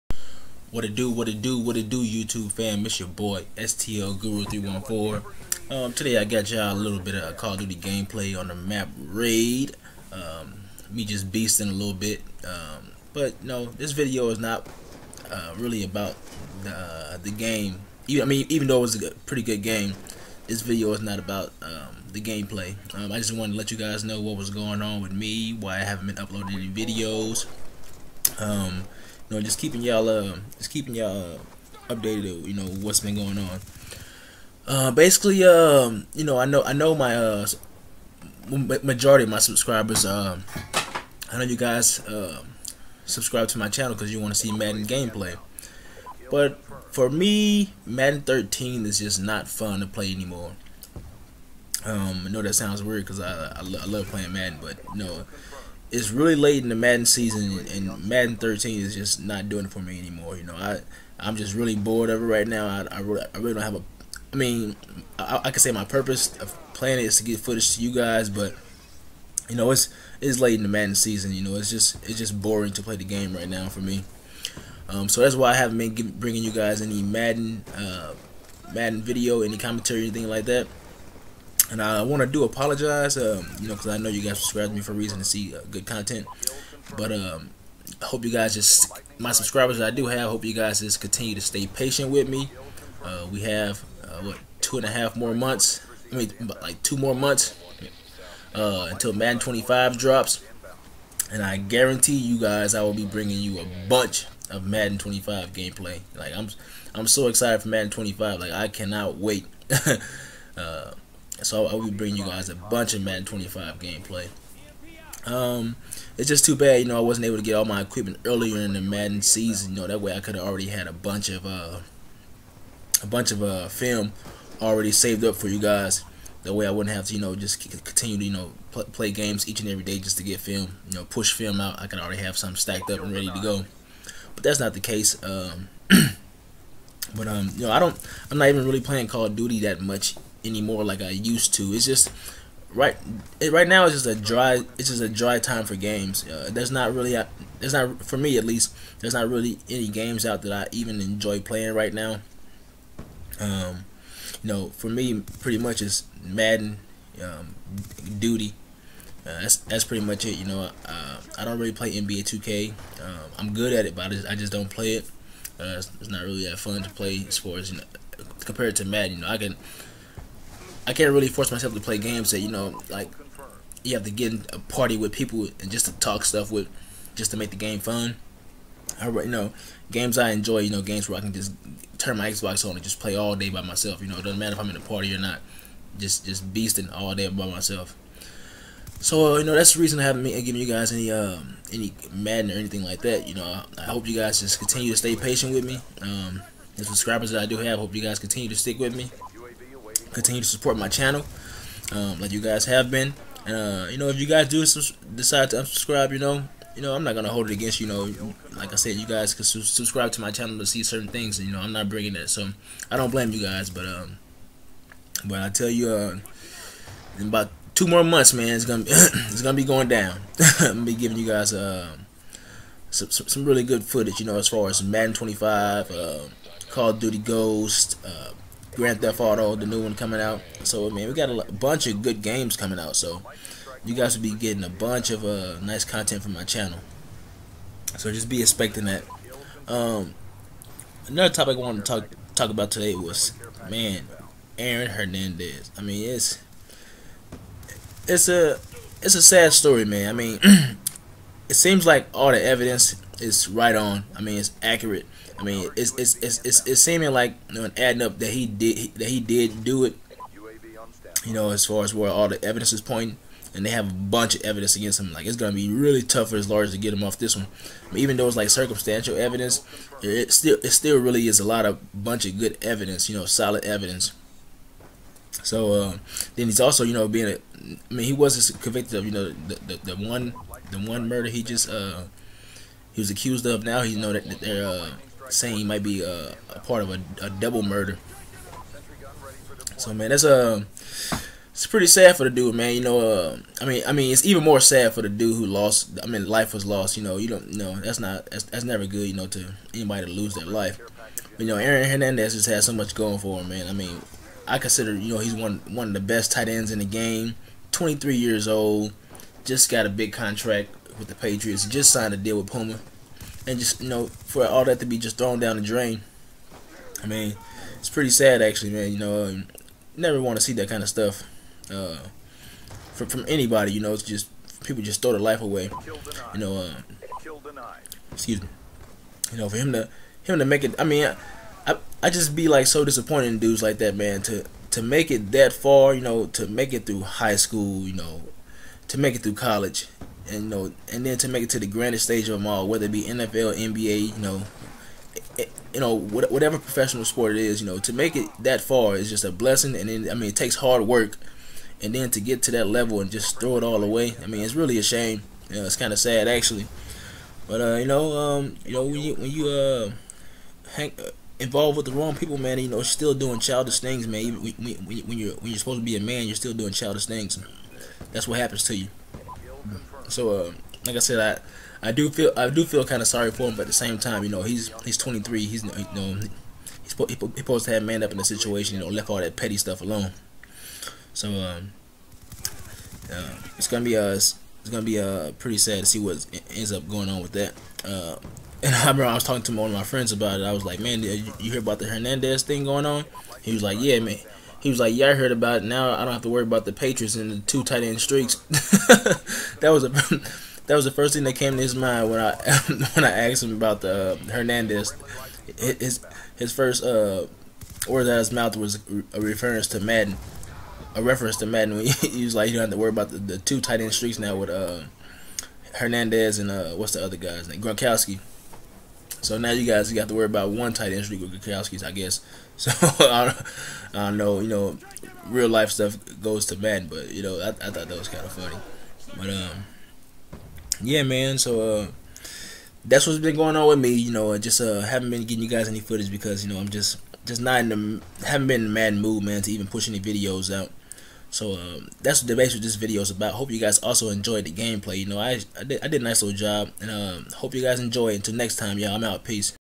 Message to SUBS!!! Enjoy the Commentary..Leave a Like!
this is just a video to let you all know whats been going on with me, and why you all havent been seeing any Madden Gameplay!